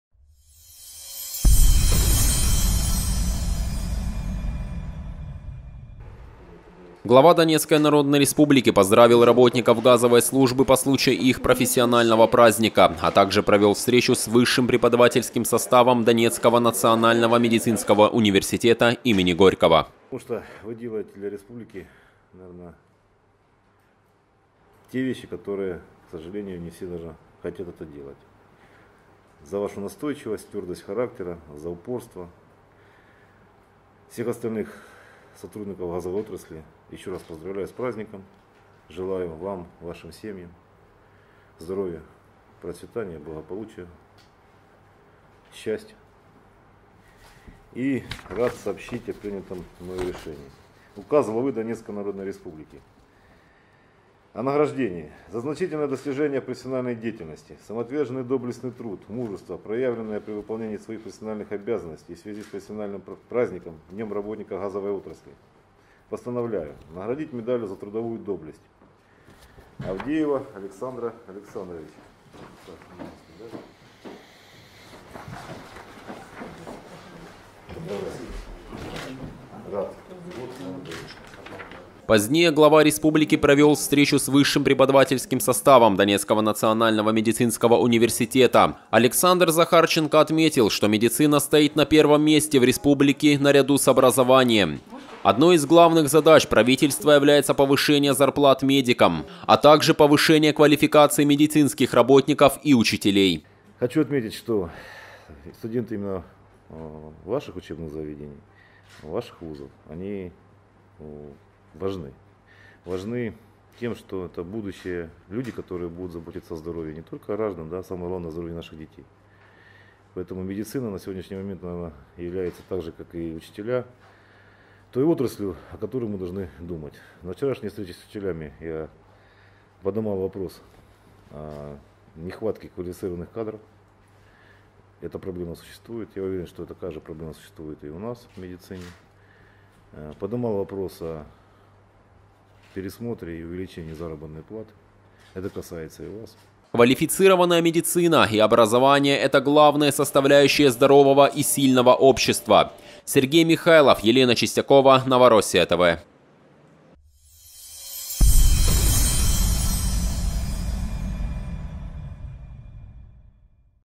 Александр Захарченко поздравил работников газовой службы
Глава ДНР поздравил работников газовой службы по случаю их профессионального праздника, а также  провёл встречу с высшим преподавательским составом Донецкого национального медицинского университета имени Горького
   Позднее глава республики провел встречу с высшим преподавательским составом Донецкого Национального Медицинского Университета. Александр Захарченко отметил, что медицина стоит на первом месте в республике, на ряду с образованием. Одной из главных задач правительства является повышение зарплат медикам, а так же повышение квалификации медицинских работников и учителей
   Квалифицированная медицина и образование это главный составляющие здорового и сильного общества. Сергей Михайлов Елена Чистякова Новороссия ТВ.